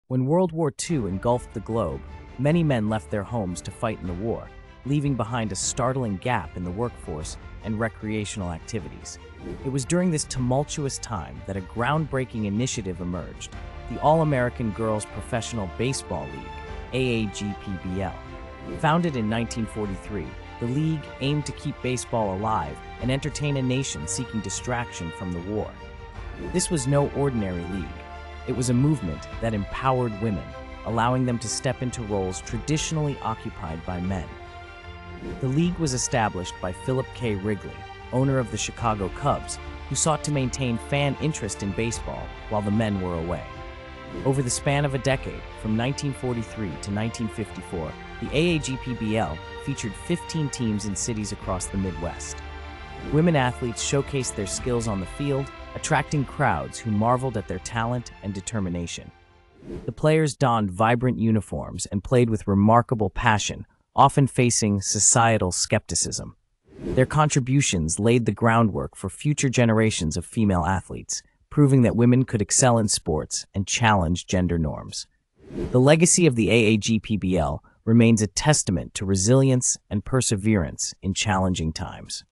The Hidden Heroes of WWII: Women Who Saved Baseball
Uncover the secrets of WWII with "The Hidden Heroes of WWII: Women Who Saved Baseball." When the world was at war, a formidable group of women stepped up to the plate, literally! Dive into the epic tale of the All-American Girls Professional Baseball League (AAGPBL), founded in 1943 by Philip K. Wrigley. Explore how these legendary women kept baseball alive and entertained a nation in need of distraction. Discover the vibrant and passionate players who defied societal norms and showcased their remarkable skills, paving the way for future generations of female athletes. Join us as we unveil the history, mystery, and legacy of these unsung heroes.